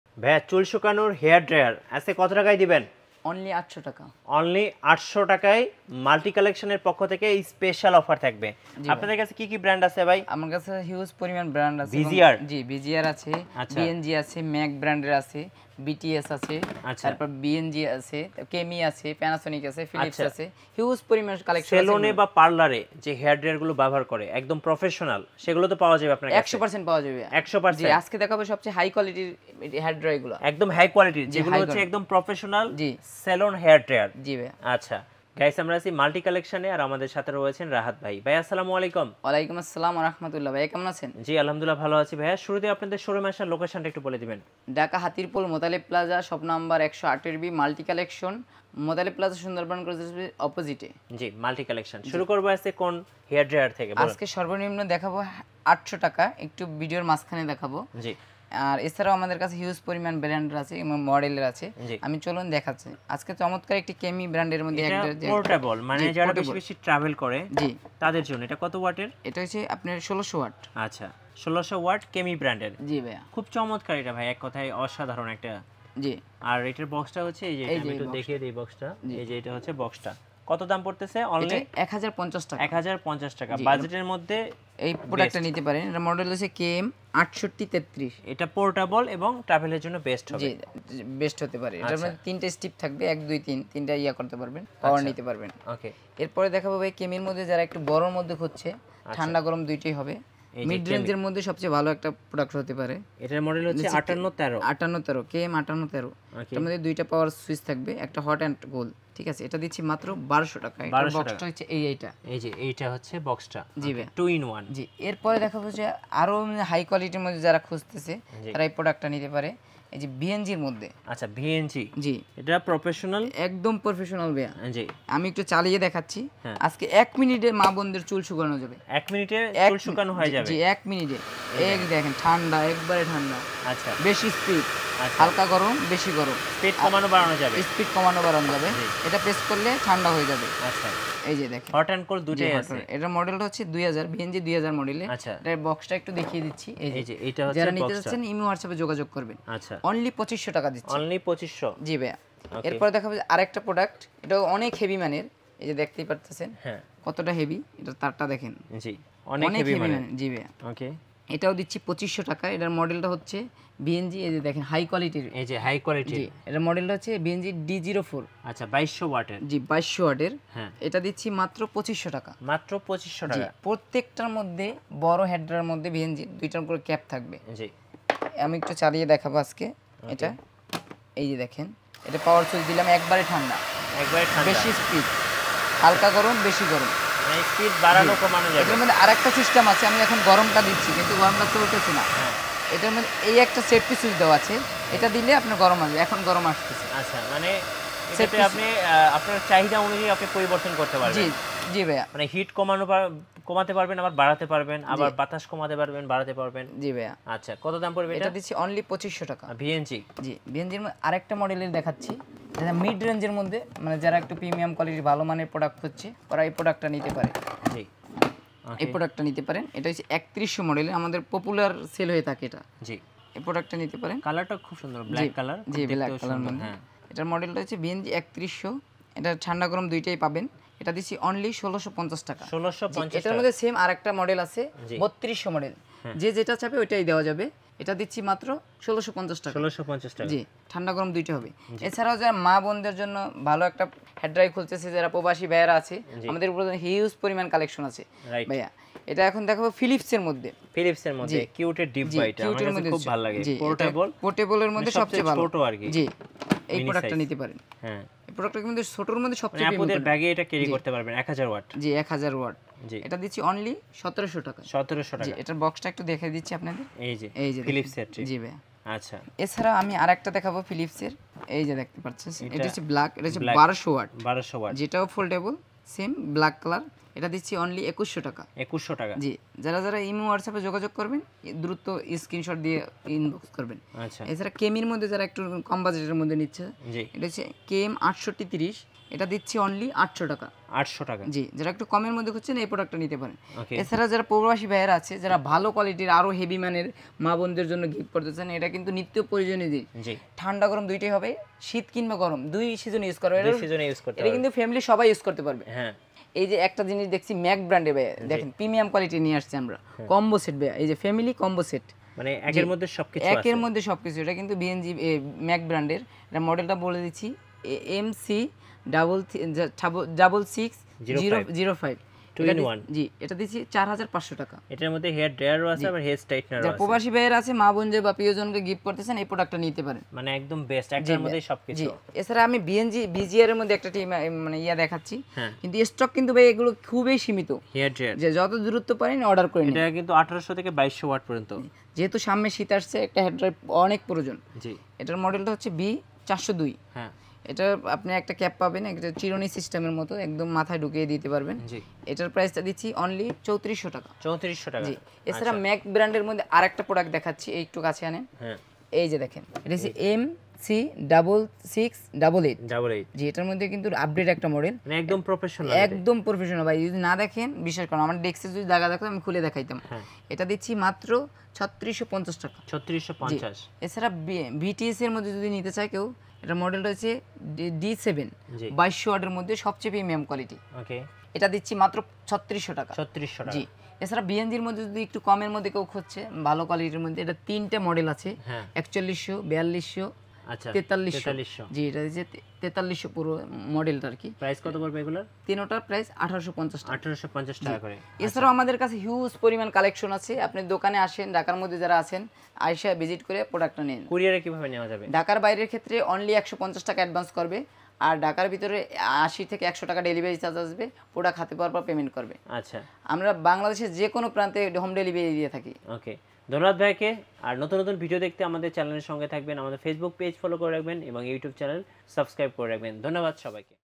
Hair dryer🔥price in bangladesh | air dryer price | hair dryer machine price in bd 2024
Multi Collection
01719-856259
Motalib Plaza Gate No:3 shop#108B/
Hatirpool, Dhaka
আপনার পণ্য বা প্রতিষ্ঠানের বিজ্ঞাপনের জন্য ই-মেইল করুন -
In this video, we explore the current prices of hair dryers in Bangladesh for 2024. Are you looking to buy a hair dryer but unsure about the cost? We've got you covered! We will discuss various brands and models available in the Bangladeshi market, along with their price ranges, features, and where to find the best deals. Whether you're a professional stylist or just someone who wants to dry their hair quickly, knowing the right price can help you make an informed decision. Don't miss out on our tips for choosing the perfect hair dryer that suits your needs and budget. Watch till the end for our top recommendations!